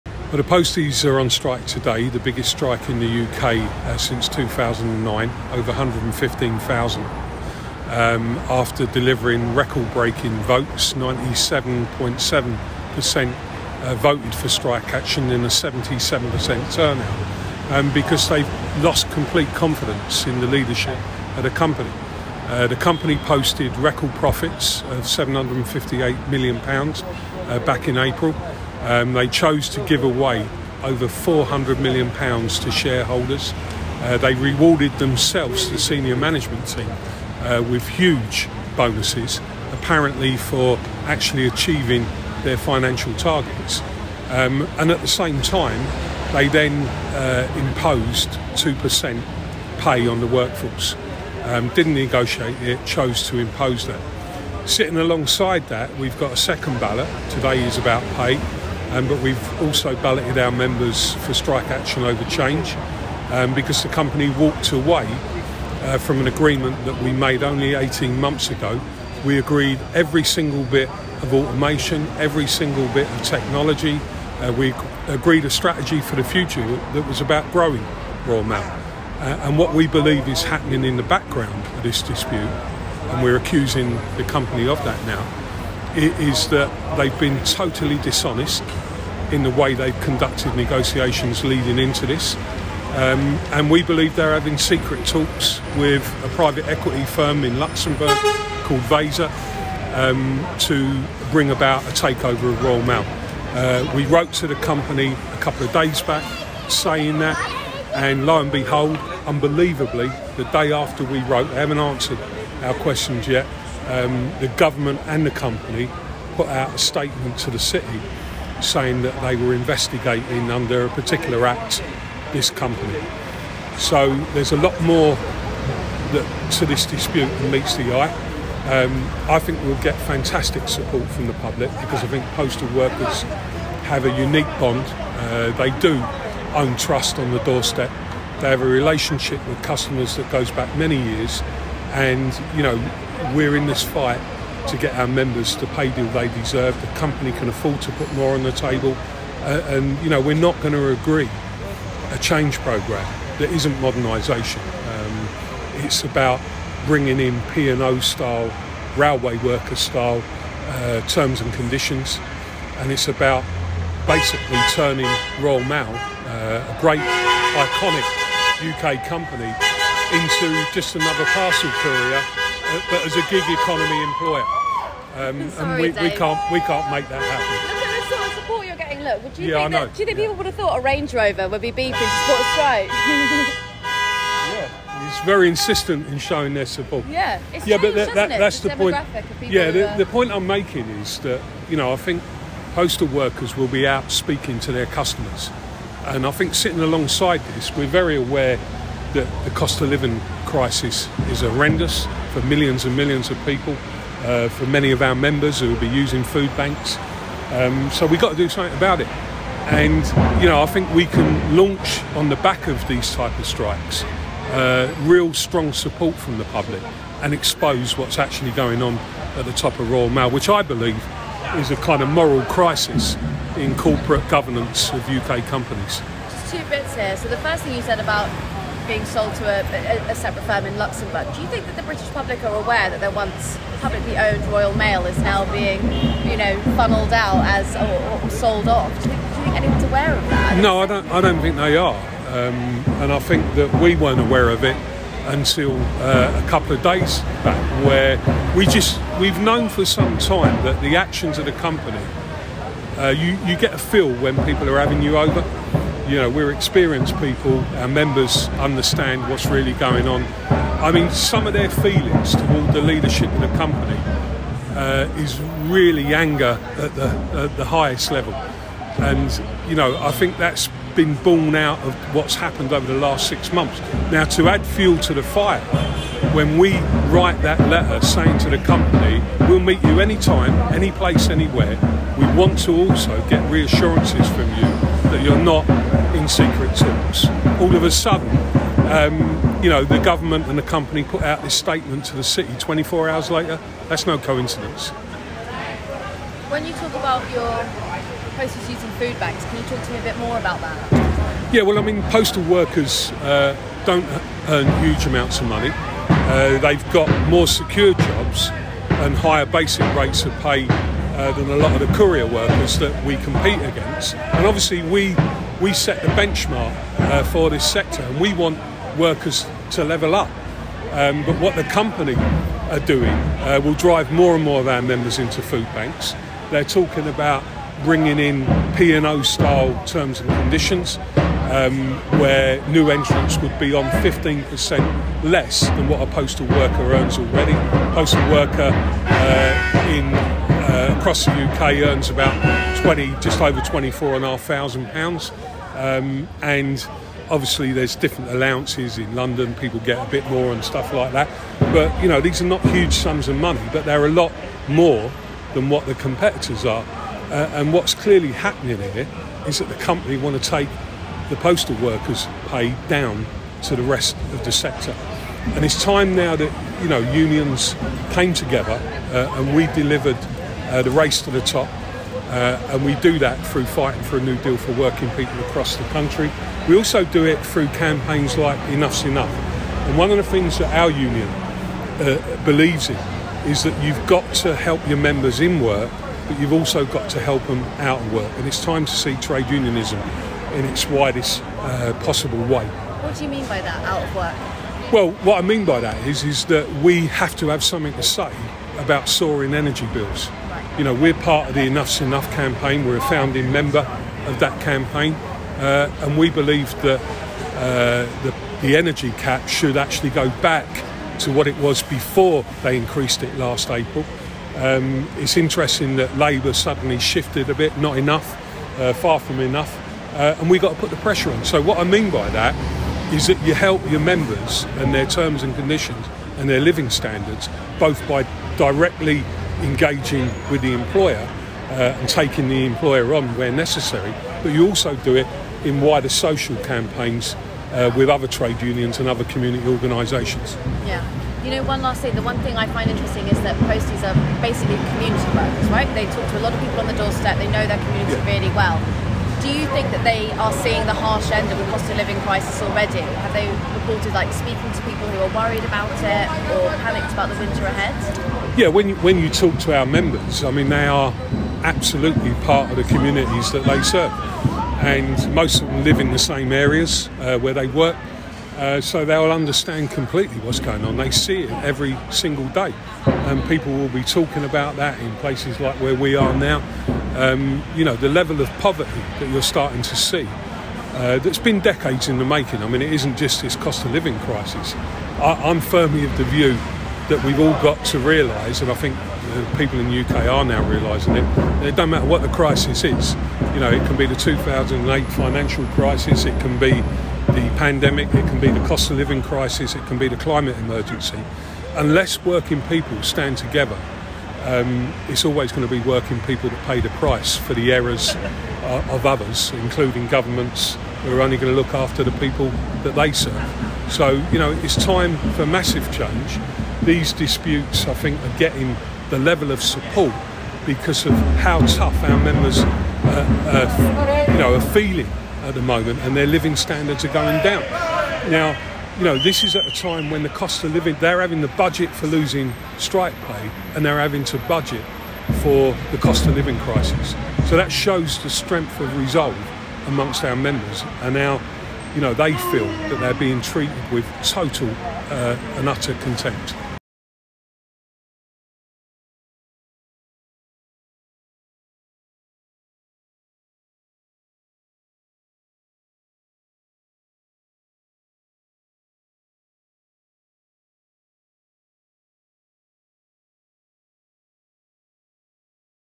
Union boss rips apart Royal Mail hypocrisy
Dave Ward of the CWU speaks to PoliticsJOE from the Whitechapel picket line.